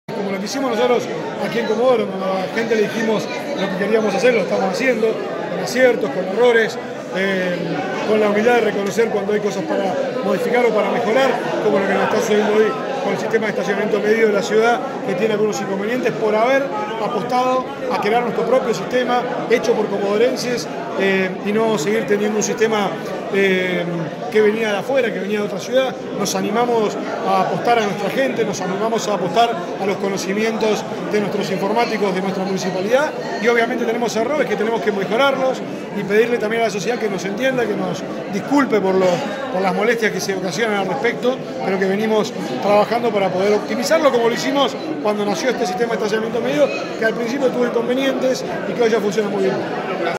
Luque reconoció las fallas en el Servicio de Estacionamiento Medido
Comodoro - Así lo manifestó el intendente de la ciudad, quien aseguró que están trabajando para mejorar el servicio.